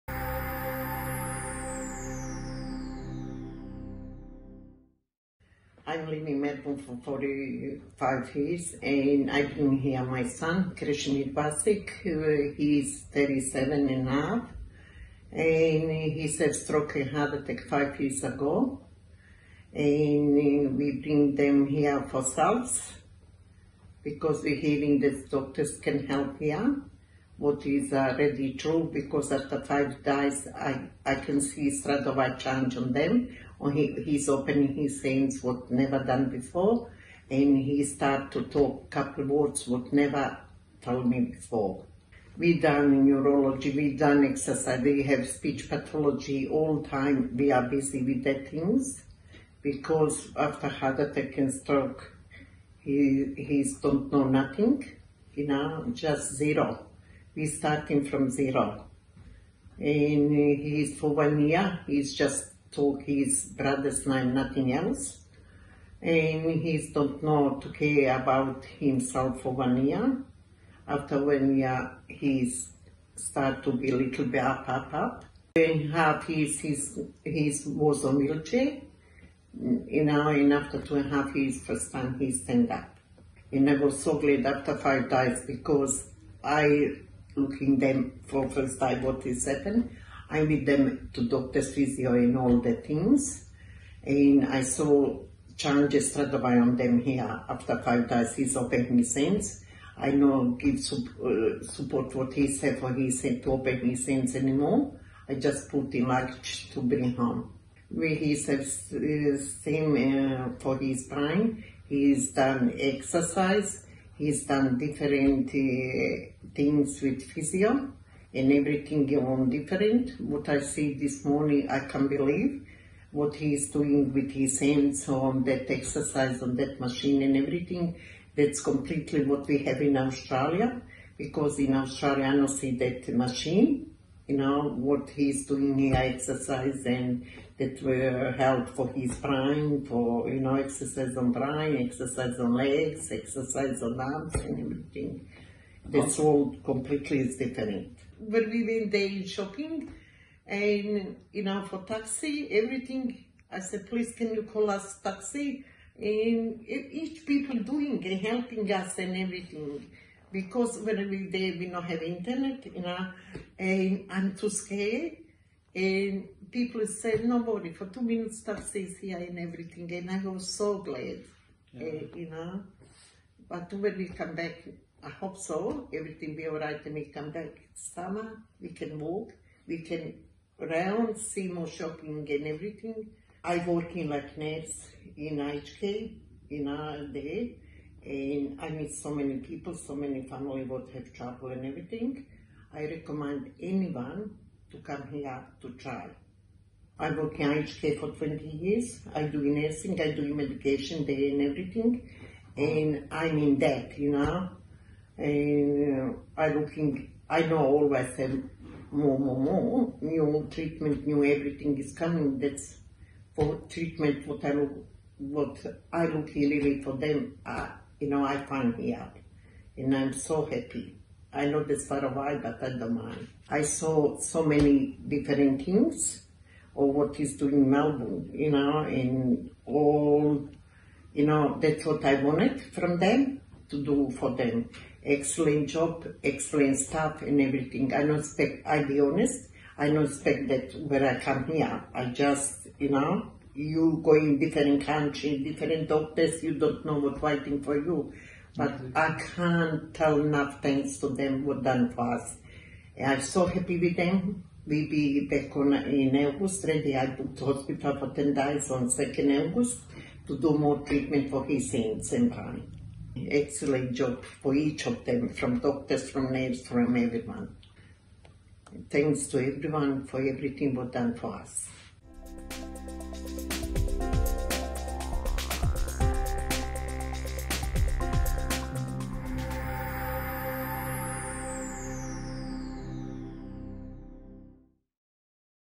Stem Cell Therapy for Post Stroke Condition
Patient from Australia, Melbourne.
Diagnosis: post-stroke and after heart attack condition for 5 years.

Before treatment:
-Left side is not functioning well
-Spasticity
-Speech difficulties
-Using a wheelchair

After treatment:
-Improved mobility
-Less spasticity
-Improved speech
-Ability to stand up and walk independently

The treatment program, 10 days:
1. Intravenous infusions of cultivated placenta-derived &amp; umbilical cord-derived
multipotent mesenchymal stromal cells (MSCs)
2. Additional therapies:
-IMR Therapy
-Super Inductive System (SIS)
-Electromyostimulation
-Xenon Gas Therapy
-Intermittent Hypoxia-Hyperoxia Therapy
-Mesodiencephalic Modulation
-Kinesiotherapy

To learn what results can be expected in your specific case, please contact our medical advisor: